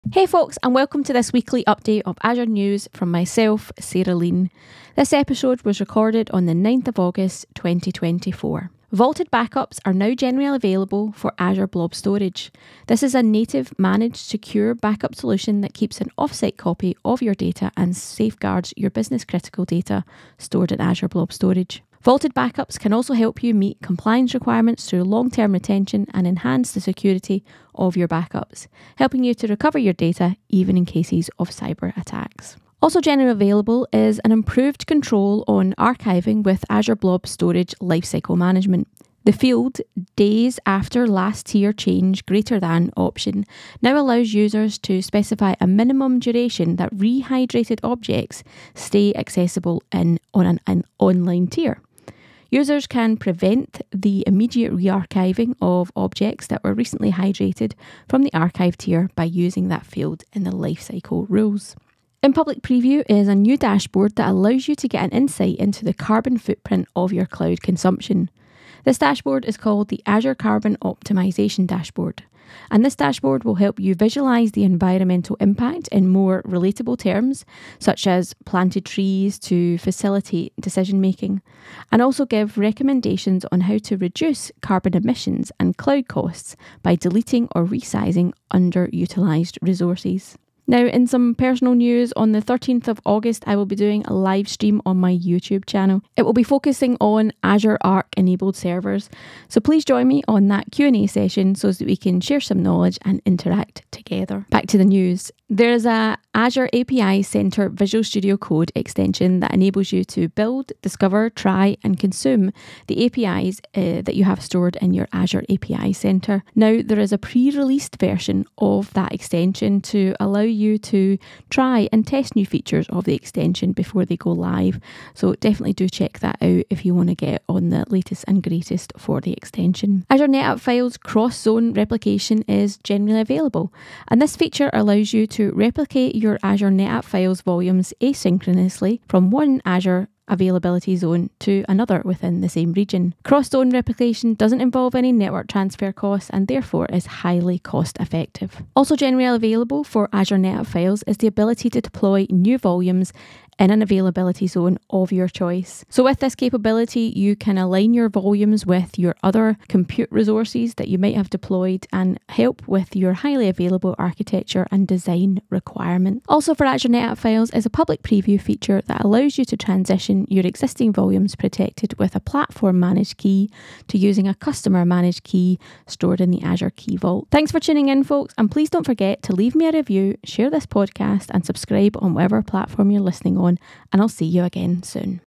Weekly Update #148 - 9th August 2024
Tune in as I explore the latest Azure updates from the past week.

Enjoyed the podcast? Consider buying me a coffee to show your support